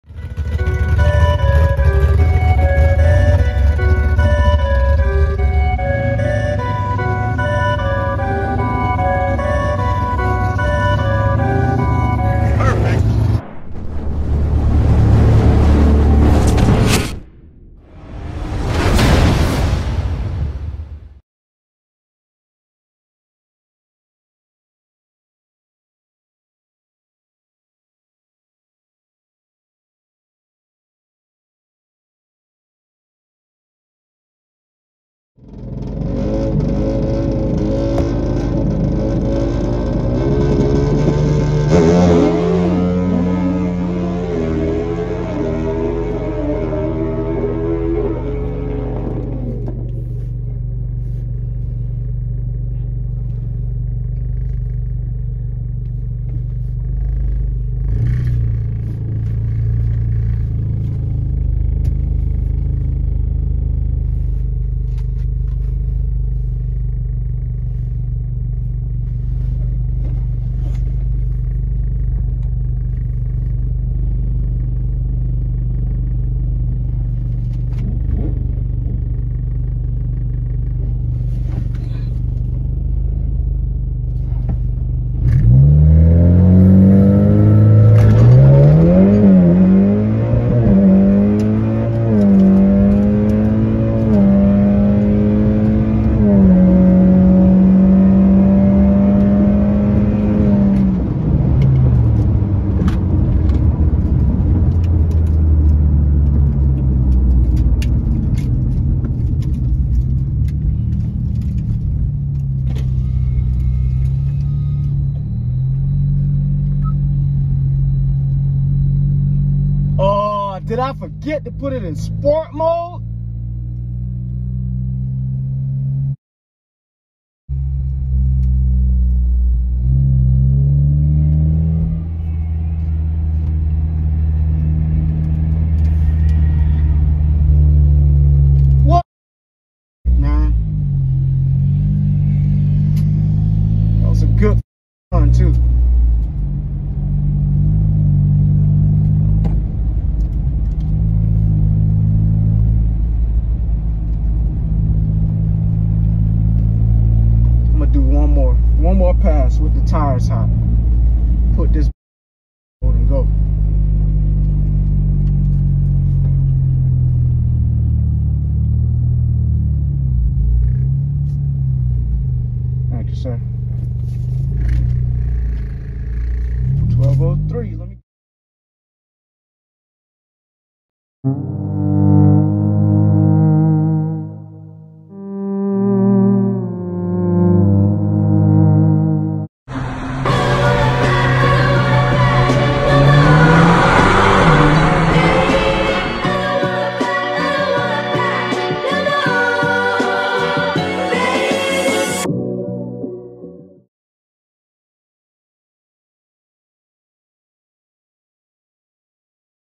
Honda Accord Sport 2.0T - First Track Day with NEW Fuel System [HD]
Took the Accord out for Test N Tune after all of my recent changes to get some data on my build with only 57% ethanol (NO METH). Not bad, but not great either.